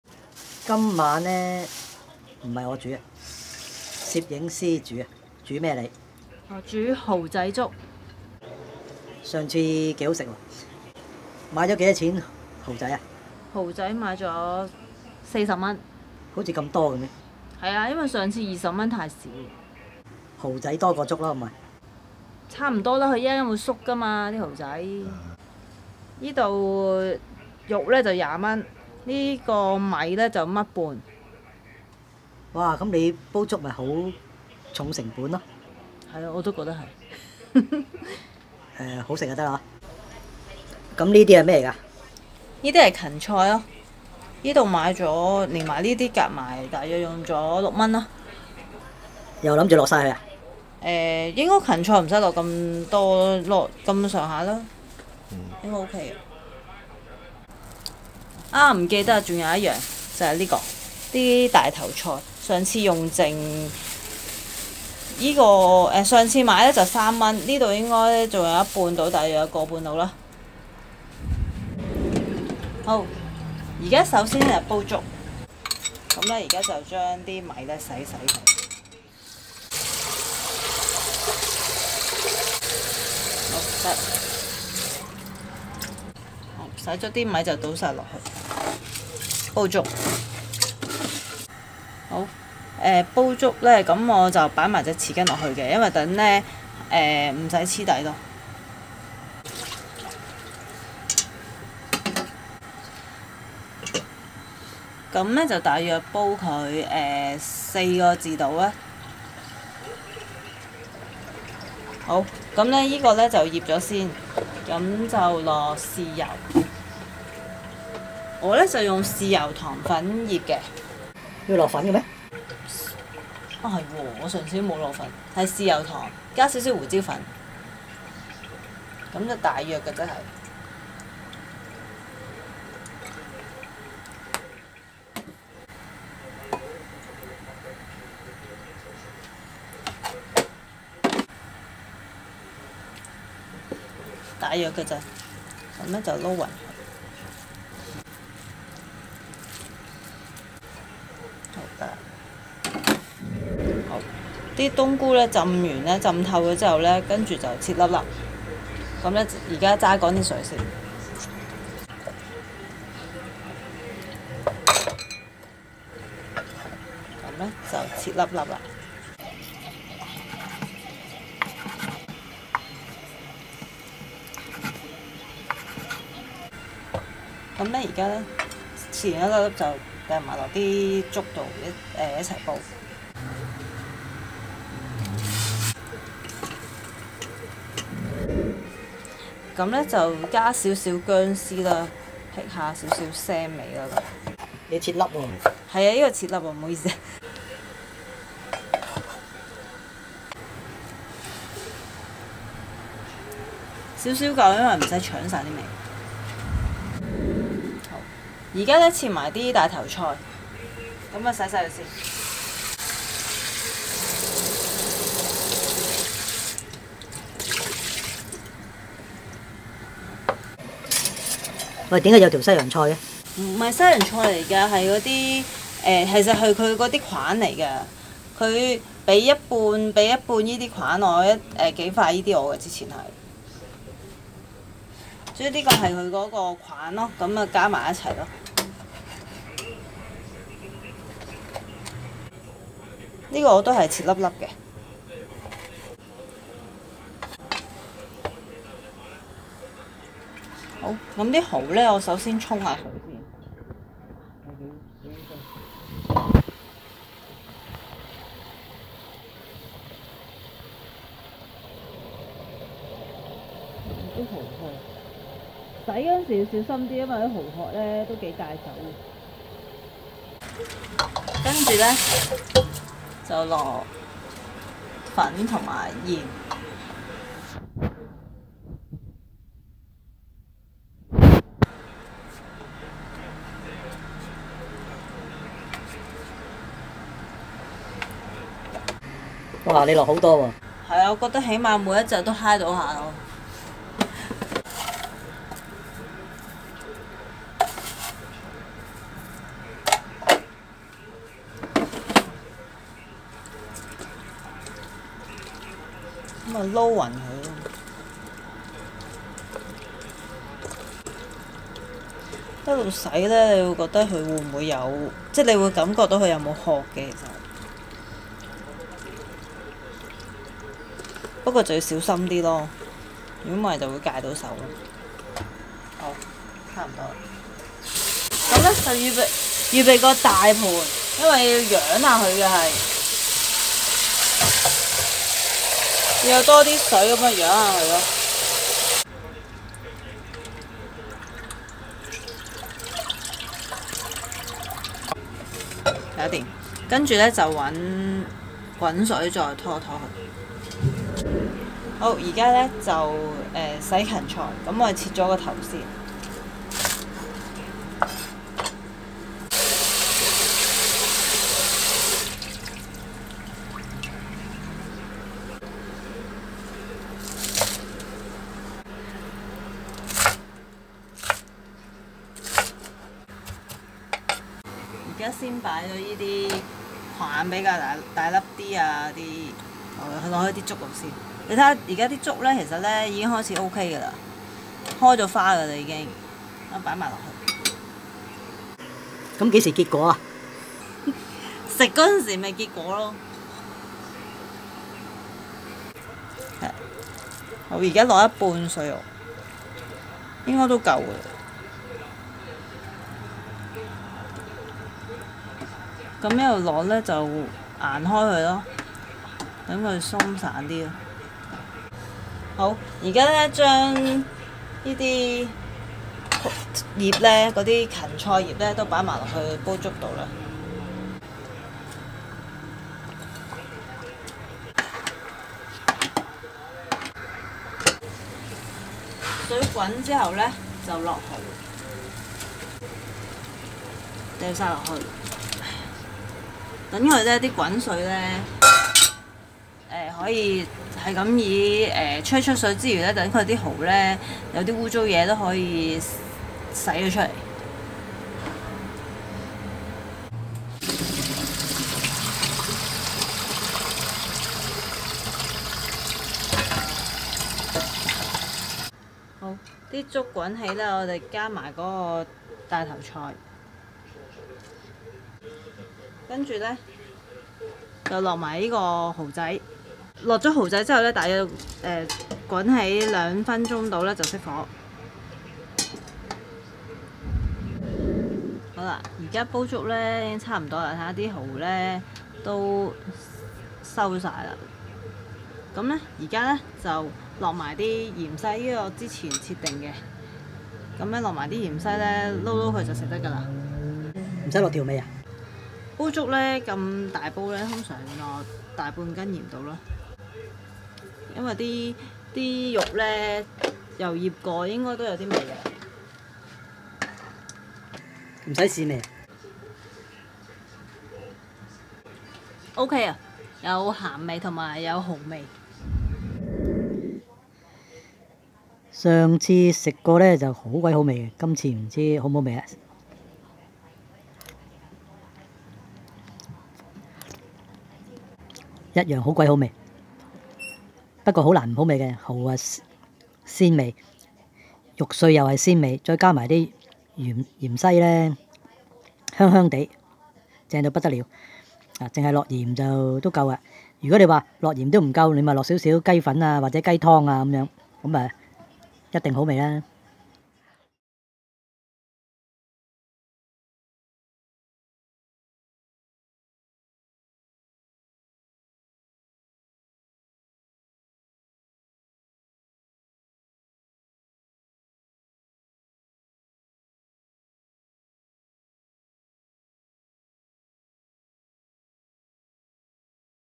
蠔仔粥 / 材料簡單，製作比想像中容易 Oyster Congee【20無限】
【20無限】的  飲食理念 ,  天下食物   唯太  X  不能救
      X =  太鹹 太甜  太酸  太苦  太辣 太硬  太軟  太多  太少  太複雜  太麻煩  太多批評 等等

材料:

蠔仔
免治豬肉
香菇
芹菜
大頭菜
芫茜